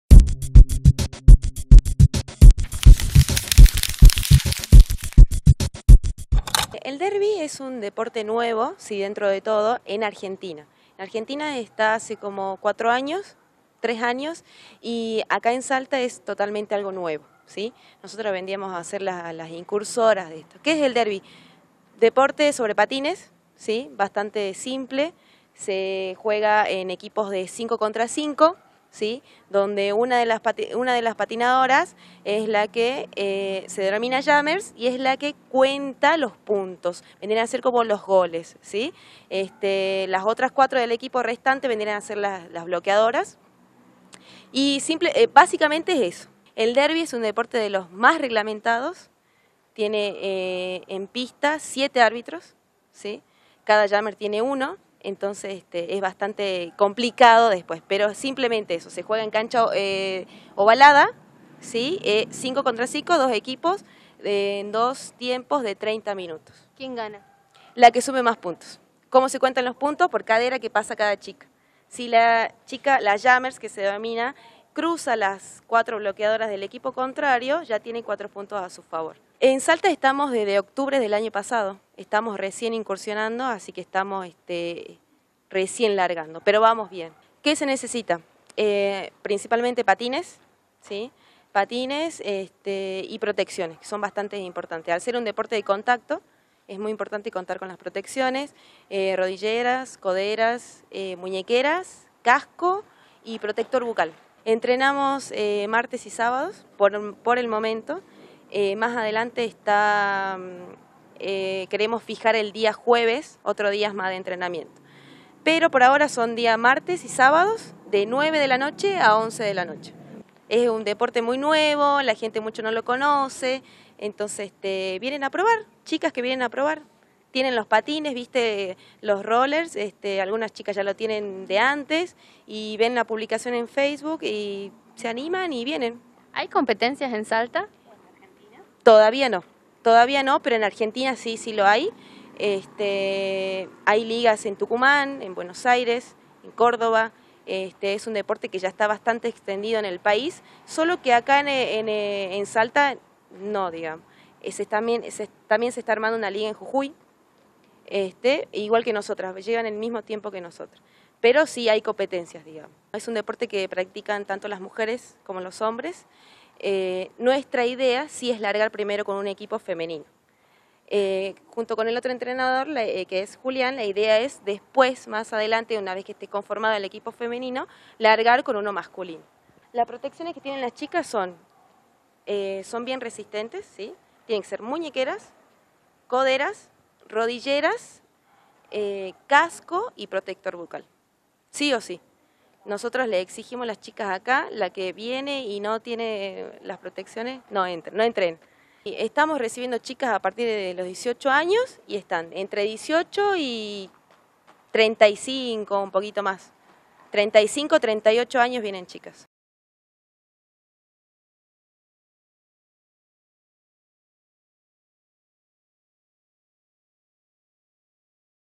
DERBY ROLLERmp4
Un deporte de contacto estadounidense que ya se entrena en Salta.